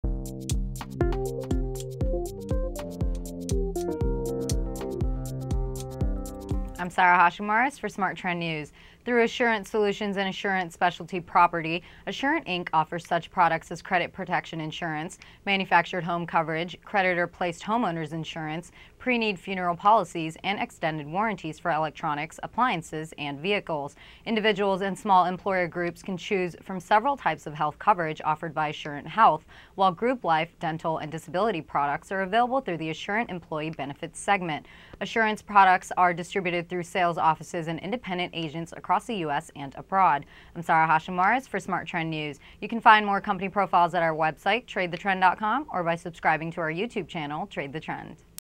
Company Profile: Assurant Inc. (NYSE:AIZ)
hrough Assurant Solutions and Assurant Specialty Property, Assurant Inc offers such products as credit protection insurance, manufactured home coverage, creditor-placed homeowners insurance, pre-need funeral policies, and extended warranties for electronics, appliances, and vehicles. Individuals and small employer groups can choose from several types of health coverage offered by Assurant Health, while group life, dental, and disability products are available through the Assurant Employee Benefits segment. Assurant's products are distributed through sales offices and independent agents across the US and abroad.